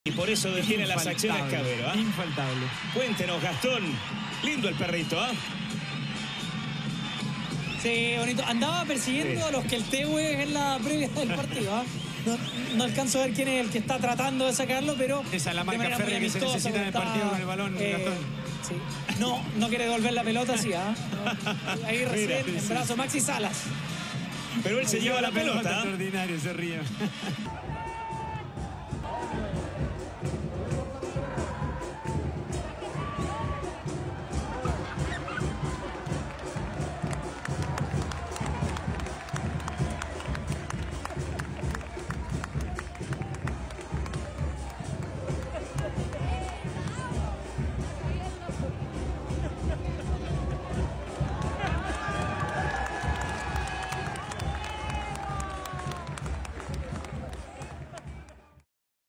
Playful pooch steals ball and hearts during professional soccer match
Fans were in for an extra treat when at a soccer in Curicó, Chile was interrupted by an unexpected visitor a playful puppy  wanted to join the game.